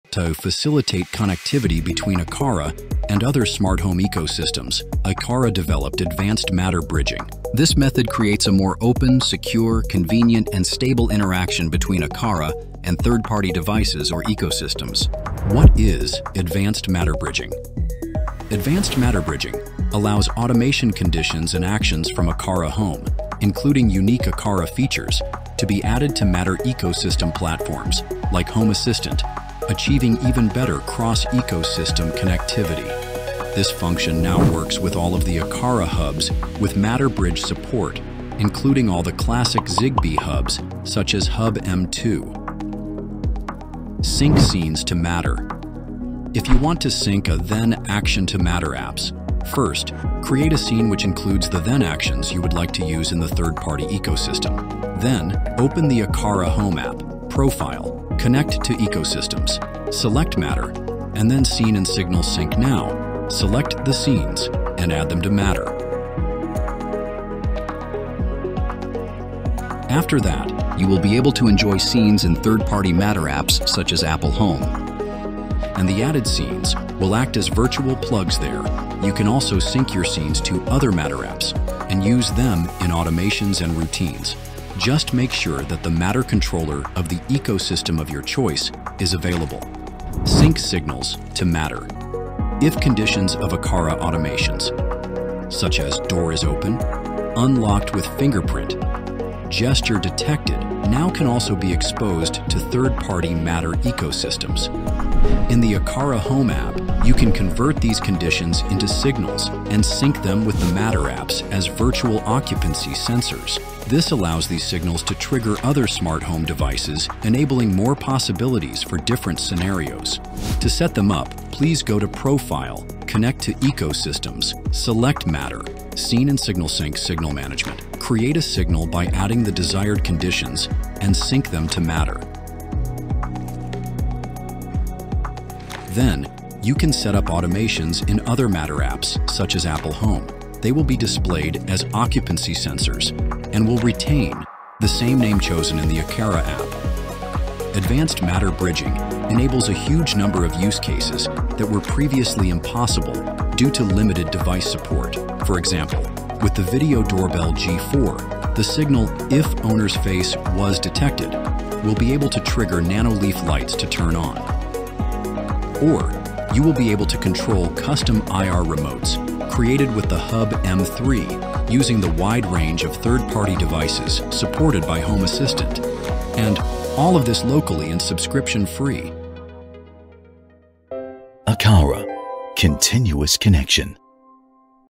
50+ new Matter devices now work with Aqara 🤯 | Plus advanced bridging is here!
Aqara Home now integrates 50+ new Matter-enabled device types, making your smart home setup more seamless and flexible. Plus, Advanced Matter Bridging is now available across all Aqara Matter Controllers, allowing you to connect Aqara's advanced features to third-party Matter platforms.

🔹 Expanded Matter Device Support – Now compatible with robot vacuums, air purifiers, EV chargers, refrigerators, water heaters, and more.
🔗 Advanced Matter Bridging – Bring Aqara's AI-powered detection, energy monitoring, and real-time triggers and actions to other Matter ecosystems like Apple Home, Google Home, Alexa, and SmartThings.
🌍 Cross-Platform Compatibility – No need to commit to a single platform—Aqara ensures your devices and automations work wherever you need them.